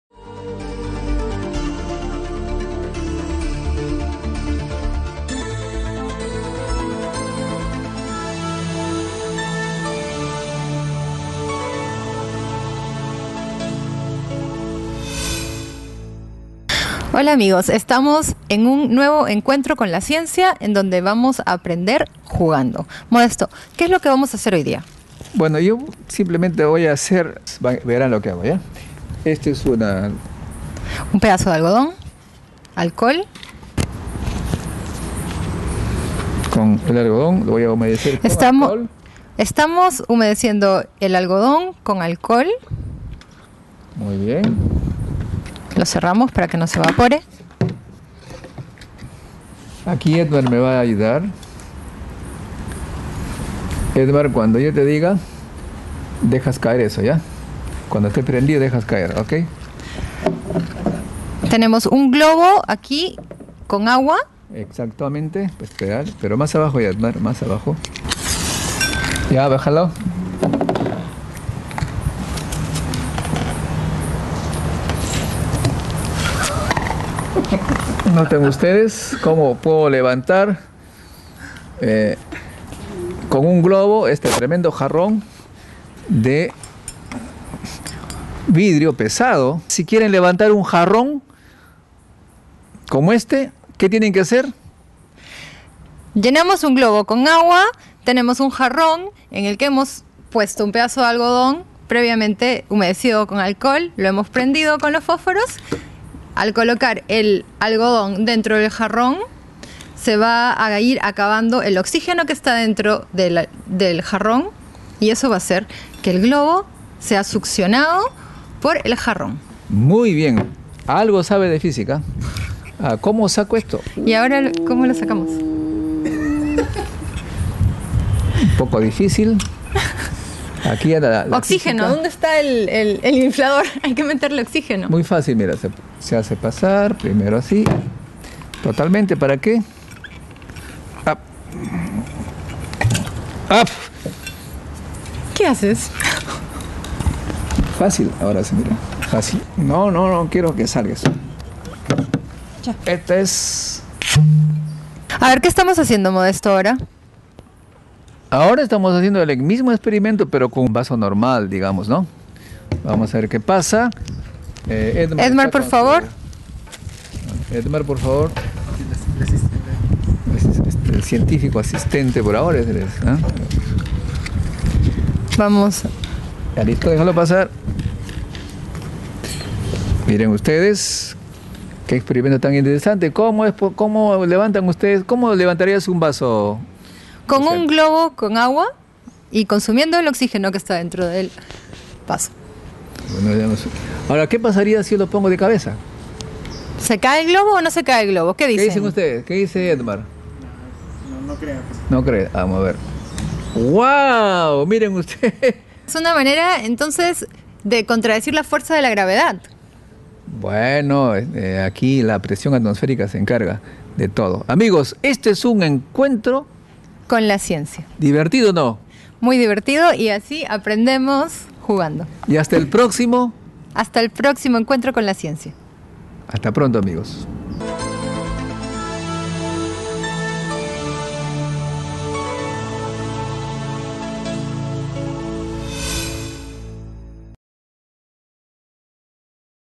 Experimento divertido ¡¿Cómo levantar un jarrón o un vaso con un globo?¡
Experimento a realizar en compañía de un adulto
Experimento divertido de presión atmosférica
¡¿Cómo levantar un jarrón o un vaso con un globo?¡
Encuentro con la Ciencia
Luciana Amado y Modesto Montoya
Director: Modesto Montoya